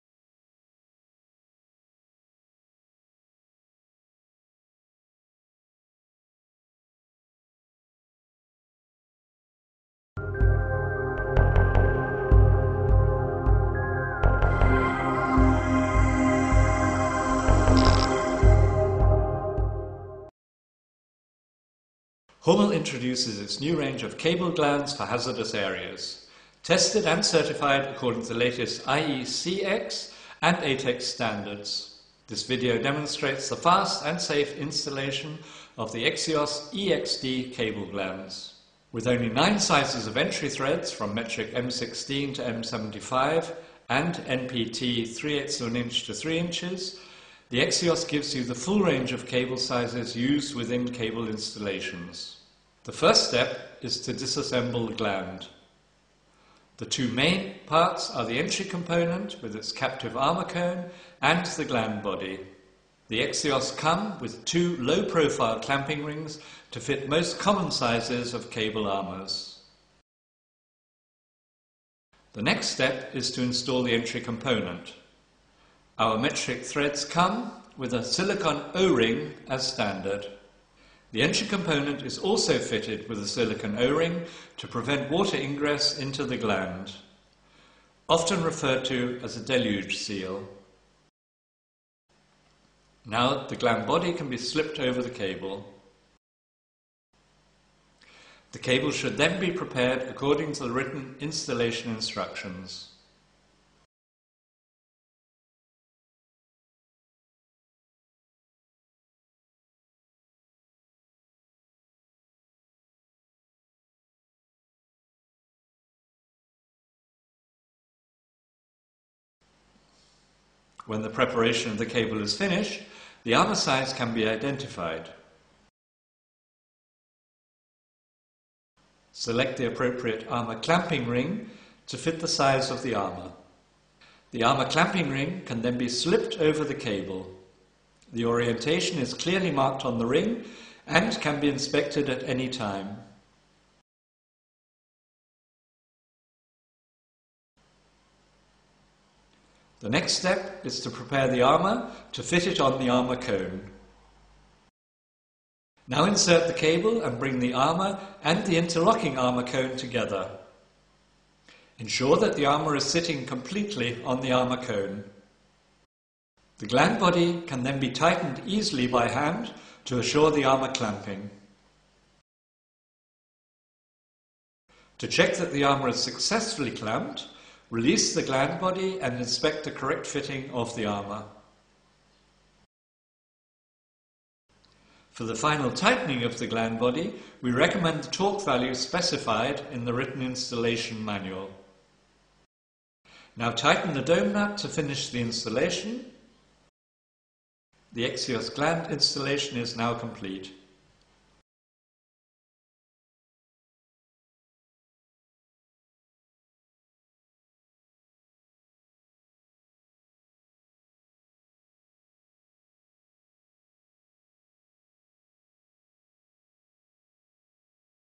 EXIOS - Cable Gland - Installation Video - HUMMEL AG
This video demonstrates an example installation of the EXIOS cable gland. In this case a SWA cable is used. Please consider, that the fast and safe installation in hazardous areas, should only be carried out by qualified personnel. It is also recommended to take note of the written installation manual.